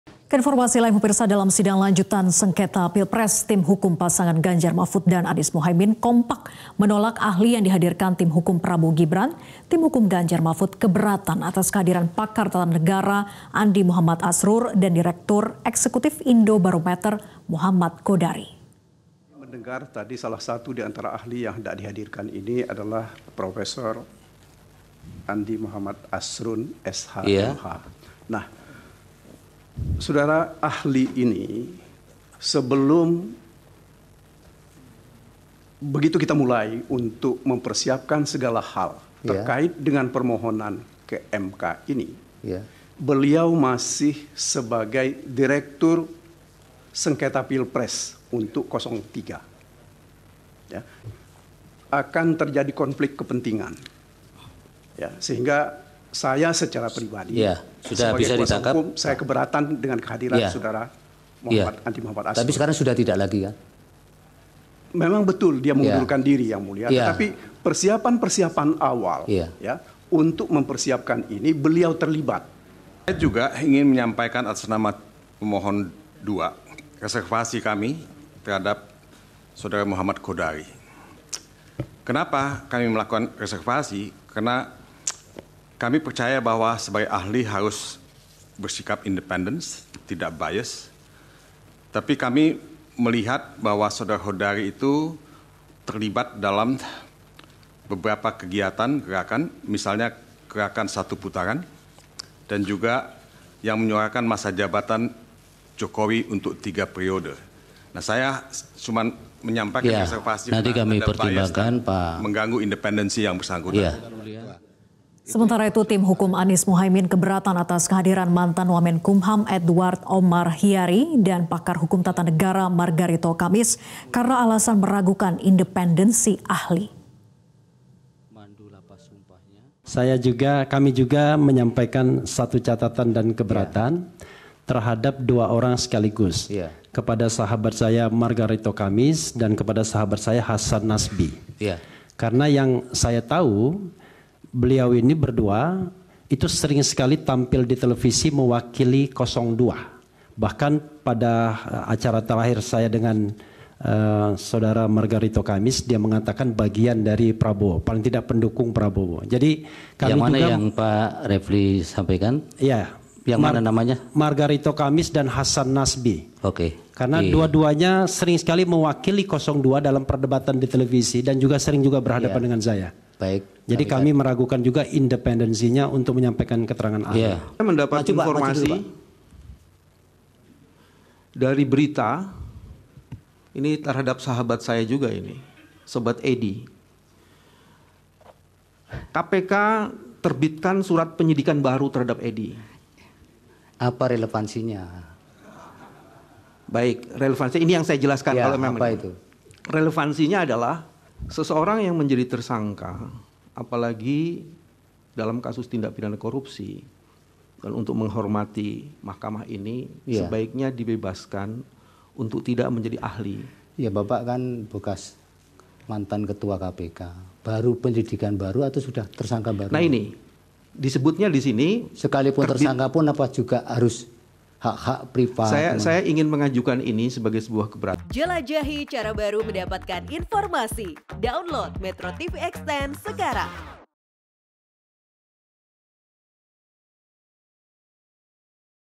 Tim AMIN Tolak Eks Wamenkumham dan Margarito Kamis
MetroTV, Dalam sidang lanjutan sengketa pilpres, tim hukum pasangan Ganjar-Mahfud dan Anies-Muhaimin kompak menolak ahli yang dihadirkan tim hukum Prabowo-Gibran.